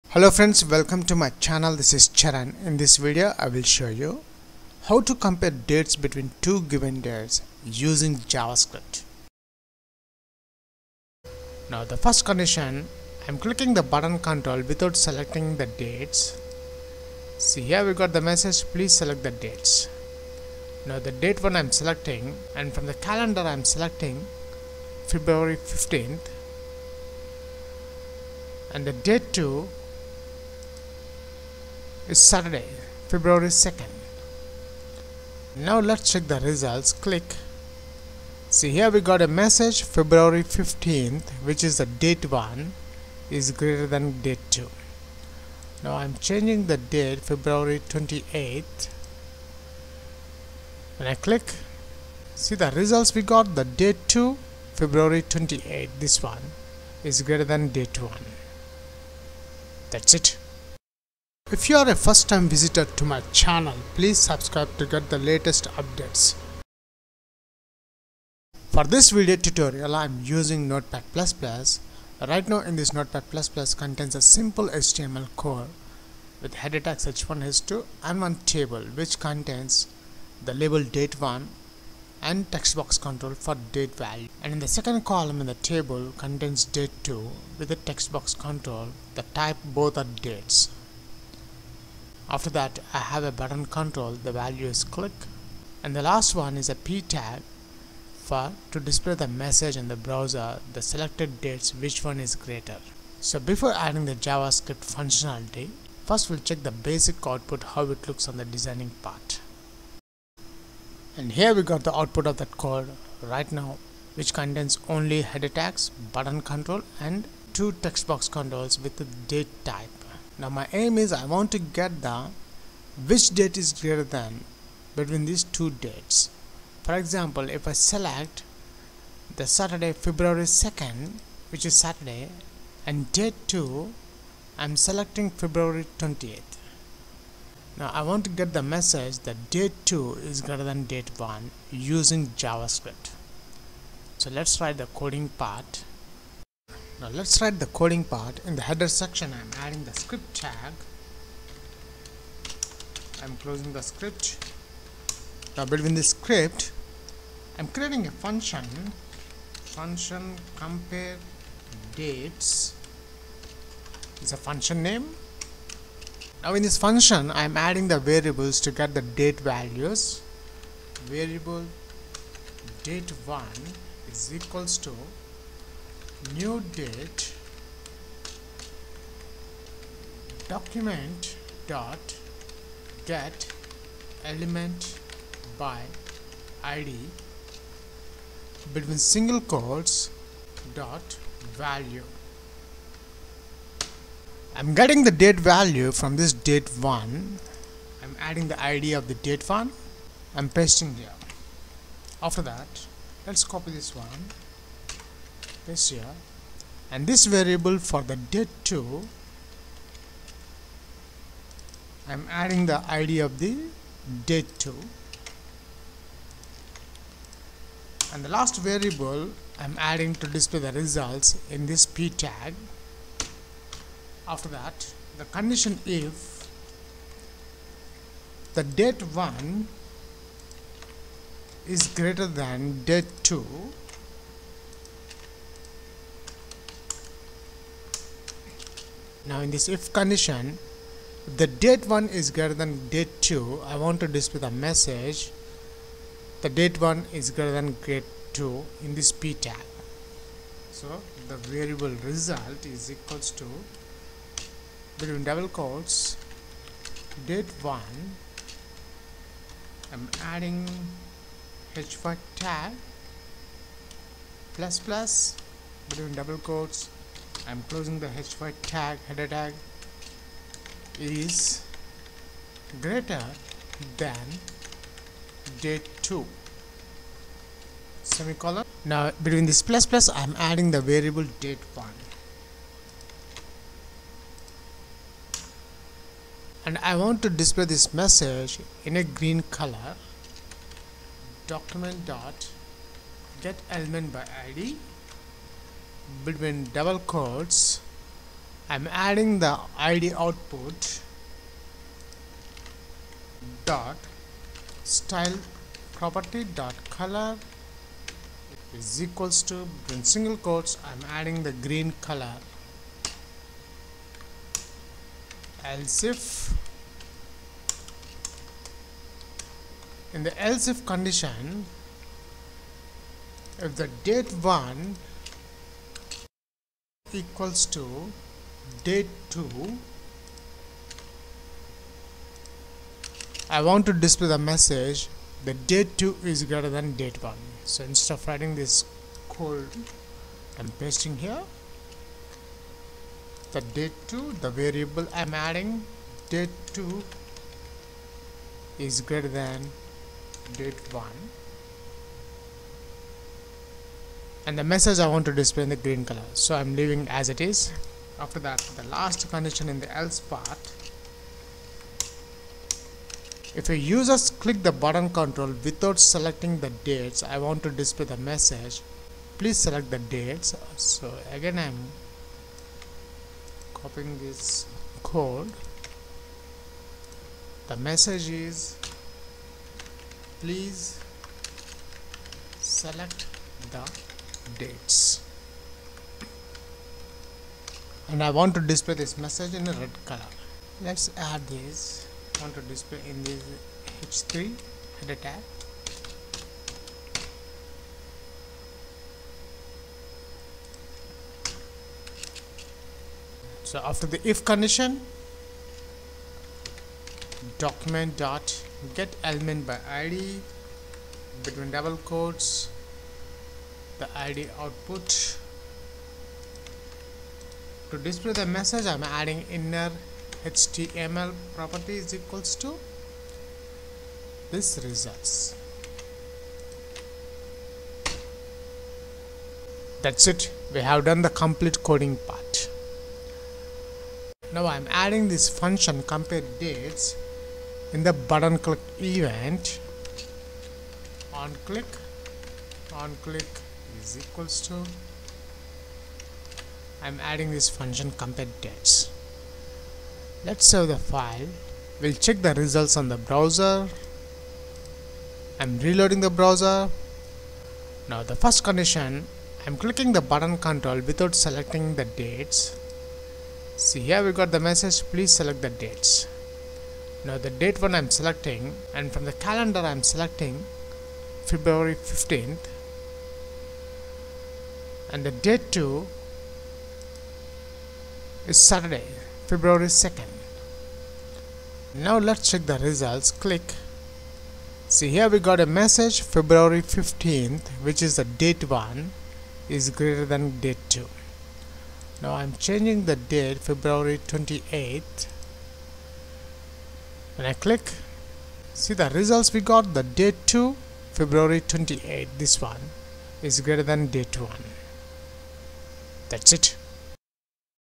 How to Compare Two Dates Using JavaScript code snippet
A Comprehensive Guide with Interview Questions | Date Object | Compare Two Dates in JavaScript. When you're working with dates in JavaScript, it's important to understand how to compare them. Whether you're creating a schedule app or dealing with time-related information, accurately comparing dates is vital. In this article, we will delve into various methods of comparing two dates using JavaScript.

Knowing how to compare dates accurately can greatly benefit your programming tasks. It allows you to determine if one date is before, after, or equal to another date. This knowledge comes in handy when organizing events, managing deadlines, or sorting data chronologically.

Within this article, we'll explore different techniques available in JavaScript for date comparison. By understanding these methods, you'll gain the ability to compare dates effectively and make informed decisions based on the results.

With the knowledge and skills acquired from this article, you'll be well-equipped to handle date comparisons in your JavaScript projects, enabling you to create more robust and efficient applications.